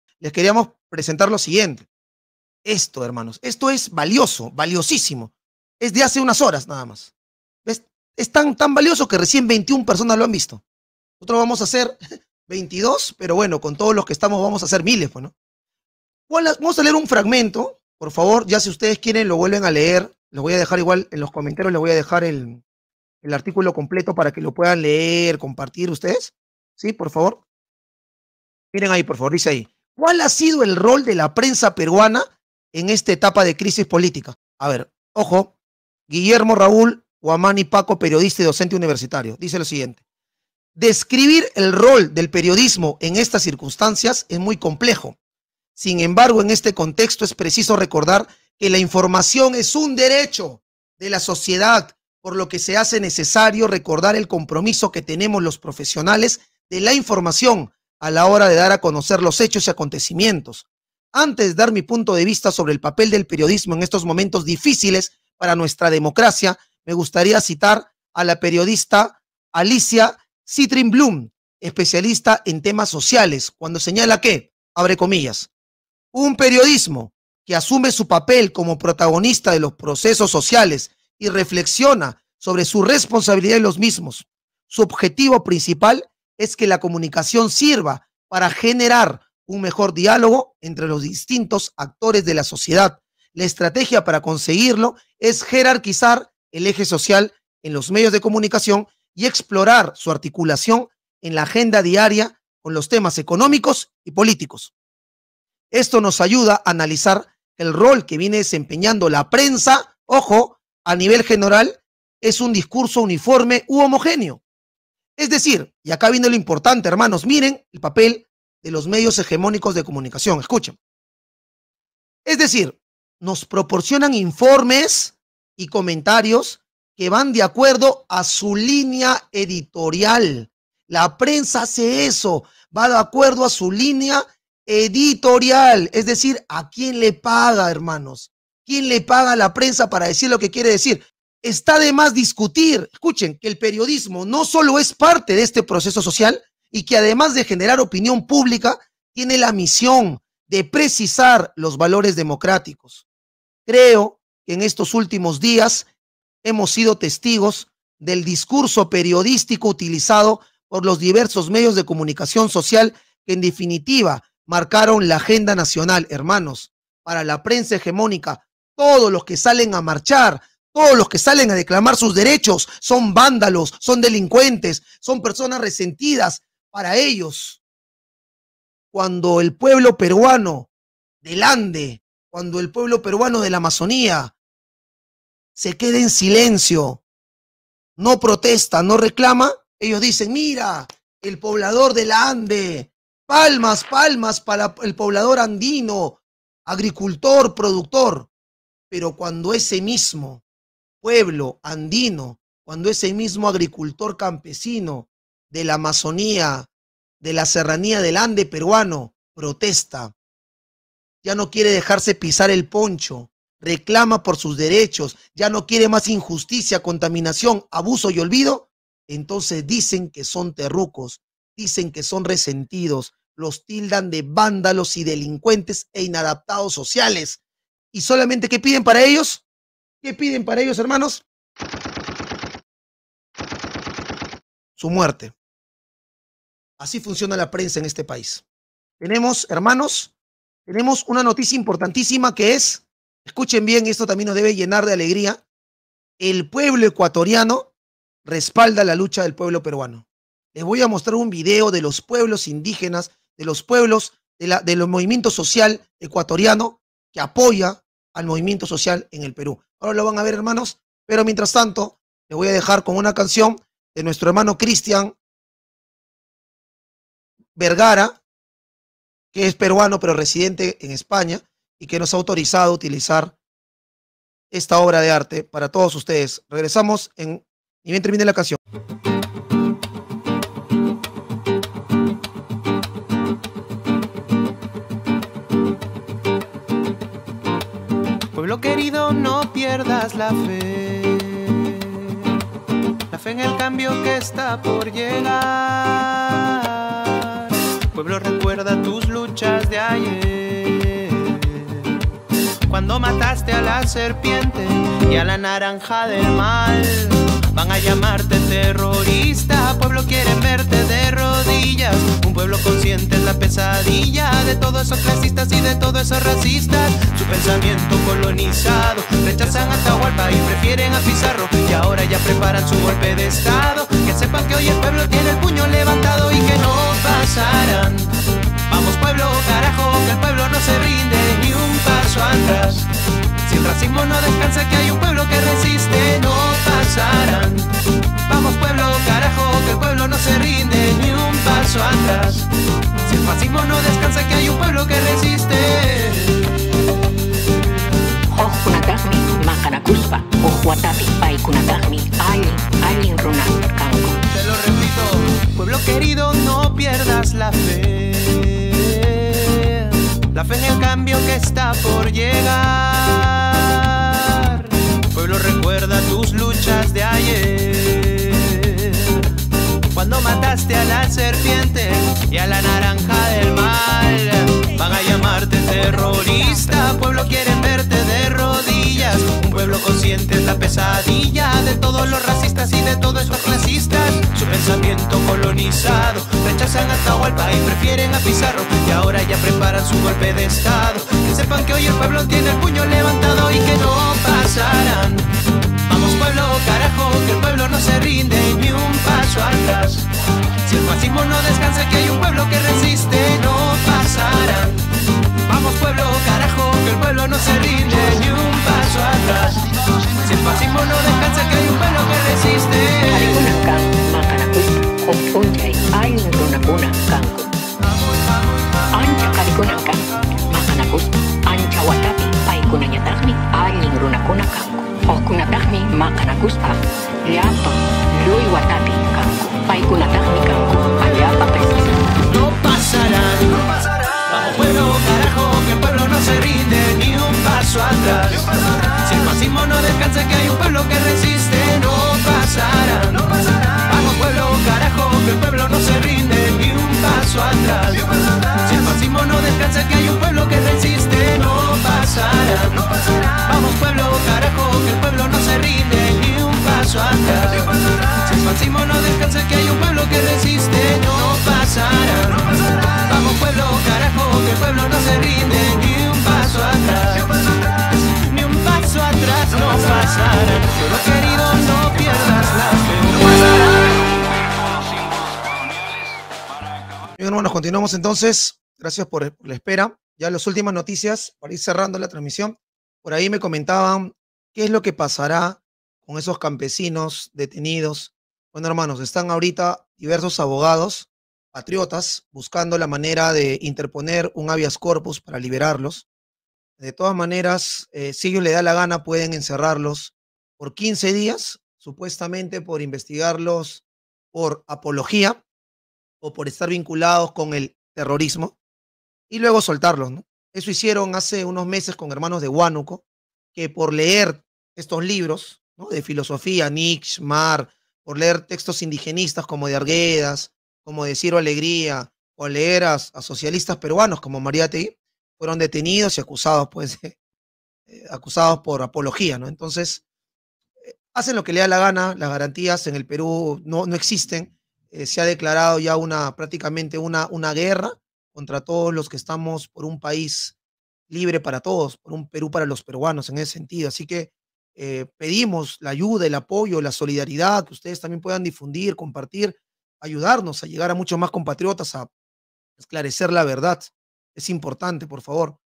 CUÁL HA SIDO EL ROL DE LA PRENSA SICARIA SOBRE EL ASESINATO A MANSALVA DE LOS HERMANOS AYACUCHANOS?
¿Cuál ha sido el rol de la prensa peruana en esta etapa de crisis política?

Guillermo Raúl Huamani Paco – Periodista y Docente Universitario

Describir el rol del periodismo en estas circunstancias es muy complejo. Sin embargo, en este contexto, es preciso recordar que la información es un derecho de la sociedad por lo que se hace necesario recordar el compromiso que tenemos los profesionales de la información a la hora de dar a conocer los hechos y acontecimientos. Antes de dar mi punto de vista sobre el papel del periodismo en estos momentos difíciles para nuestra democracia, me gustaría citar a la periodista Alicia Cytrynblum, especialista en tema sociales, cuando señala que “… un periodismo que asume su papel como protagonista de los procesos sociales y reflexiona sobre su responsabilidad en los mismos. Su objetivo principal es que la comunicación sirva para generar un mejor diálogo entre los distintos actores de la sociedad. La estrategia para conseguirlo es jerarquizar el eje social en los medios de comunicación y explorar su articulación en la agenda diaria con los temas económicos y políticos “.

Video: PávelYachay
Canción: Cristian Vergara